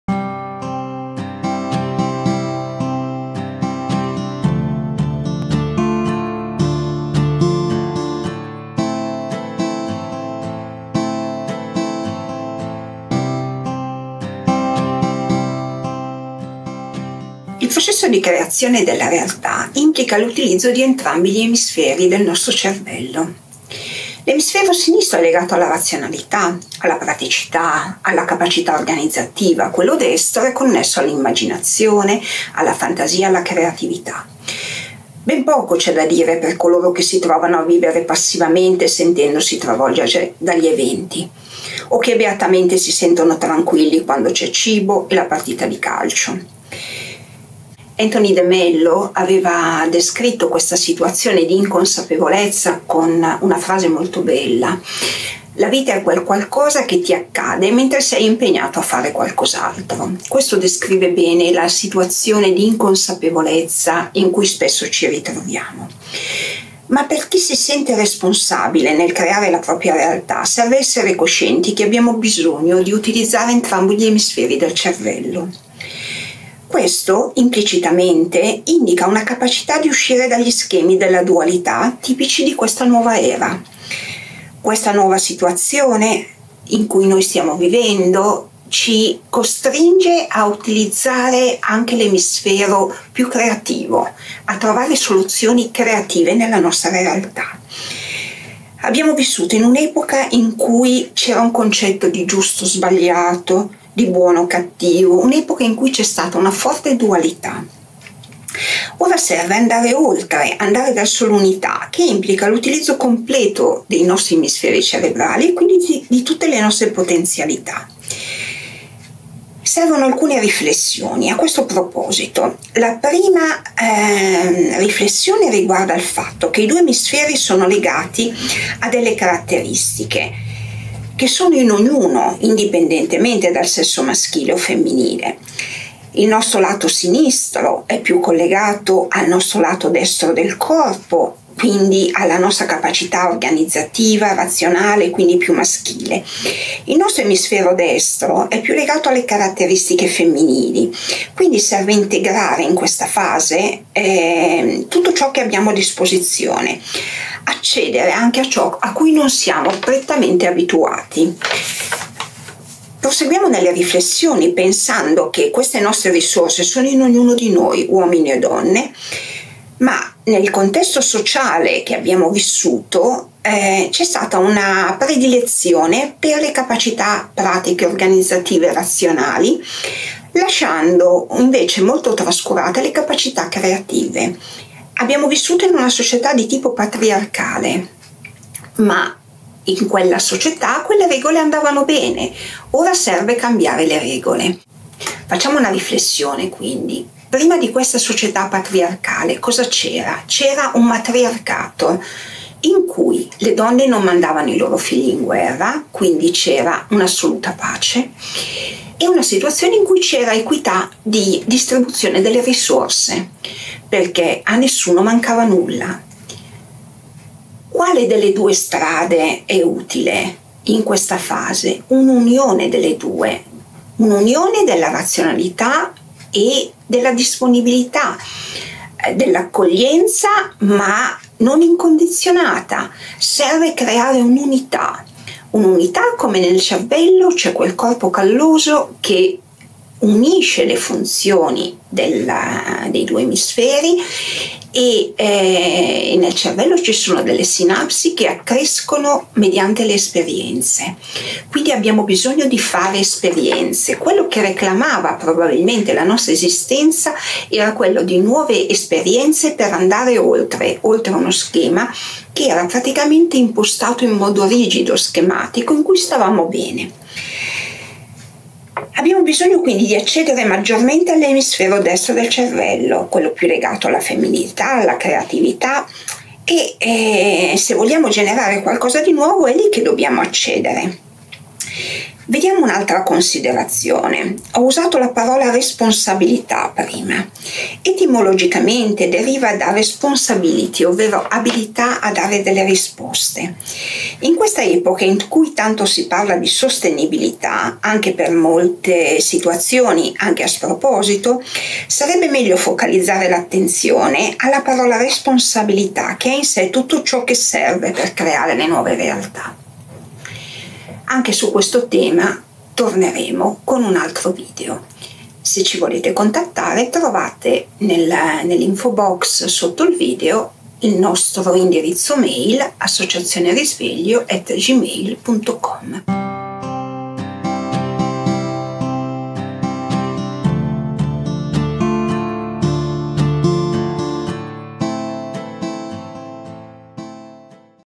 creare con entrambi gli emisferi
Utilizziamo entrambi gli emisferi cerebrali per creare la nostra realtà? Riusciamo ad accedere sia alla parte razionale e organizzata dell'emisfero sinistro del cervello che a quella istintiva e creativa dell'emisfero destro? Siamo esseri completi o viviamo nella dualità? Ecco alcune riflessioni. E seguirà un video specifico su come accedere maggiormente alla nostra parte creativa, usando l'emisfero più adatto per questa funzione.